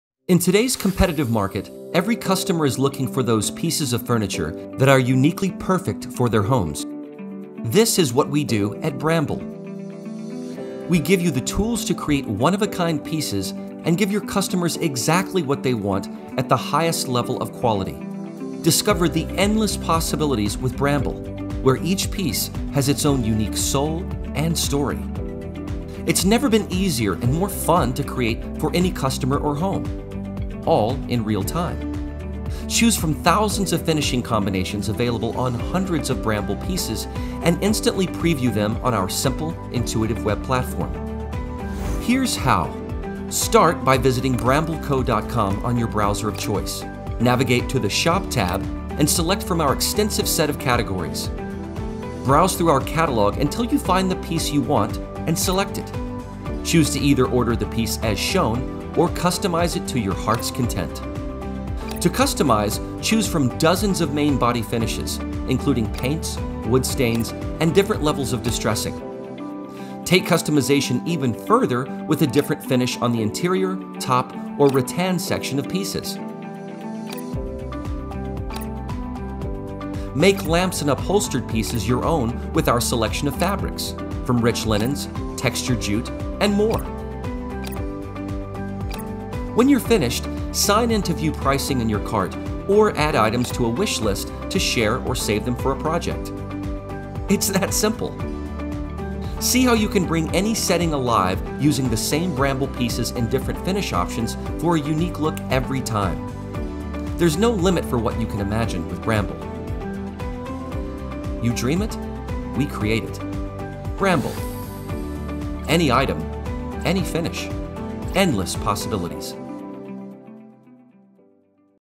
Any Item. Any Finish. Endless Possibilities | Customization Walkthrough | Bramble
It’s never been easier and more fun to create unique furniture for any home – all in real time. Choose from thousands of finishing combinations available on hundreds of Bramble pieces and instantly preview them on our simple, intuitive web platform.